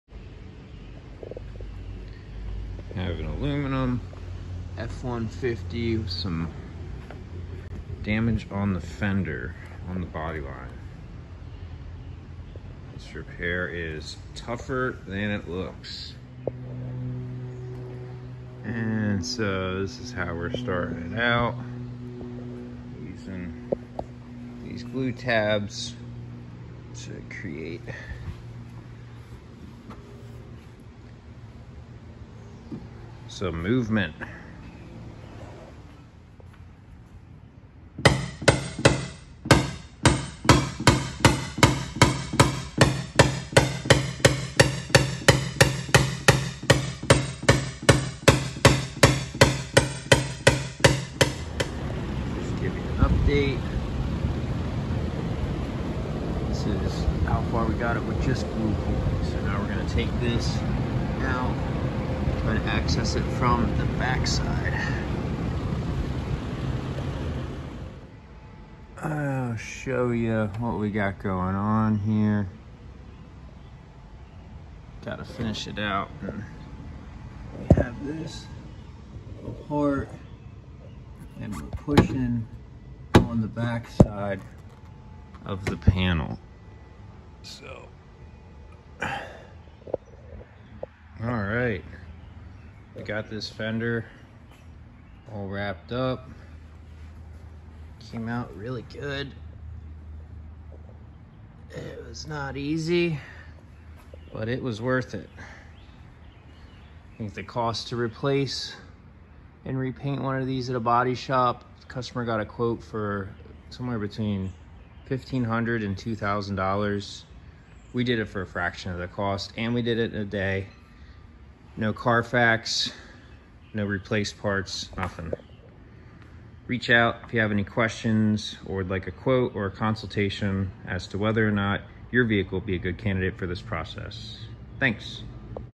Aluminum Ford Truck Dent Repair
We often get asked if we are able to repair ALUMINUM body panels.  The answer is yes! While it is more difficult, you can expect great results IF you choose the right technician.  Paintless Dent Removal is an art, make sure you do your homework.  Checkout our website for more info or to get a quote.